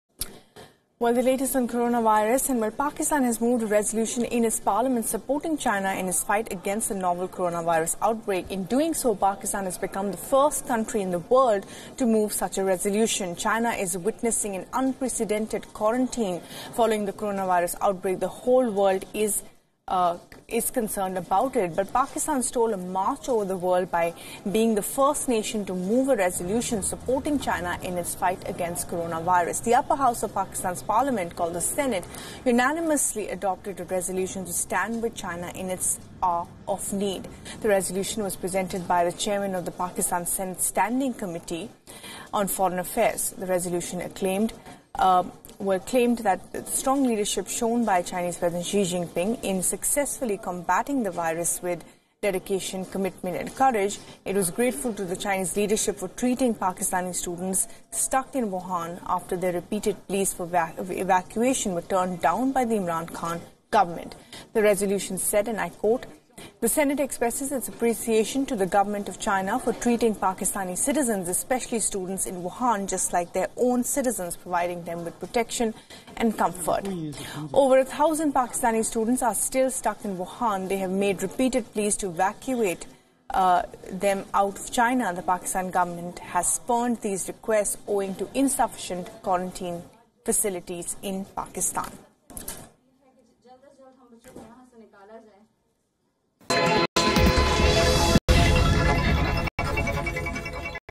Pak Senate steps in support of China, Passes resolution praising China's fight against Coronavirus
Pakistan has moved a resolution in its Parliament supporting China in its fight against the deadly coronavirus outbreak. Doing so Pakistan has become the first nation in the world to move such a resolution. China is witnessing an unprecedented coroutine following the coronavirus outbreak. The whole world is concerned about it. The upper house called the Senate of Pakistan parliament has unanimously adopted a resolution to stand with China in its hour of need.

WION: shorturl.at/fwKO0
Zee News English: shorturl.at/aJVY3
Zee News Hindi: shorturl.at/eorM1
Zee Business: shorturl.at/hpqX6
DNA News: shorturl.at/rBOY6
BGR: shorturl.at/eioqL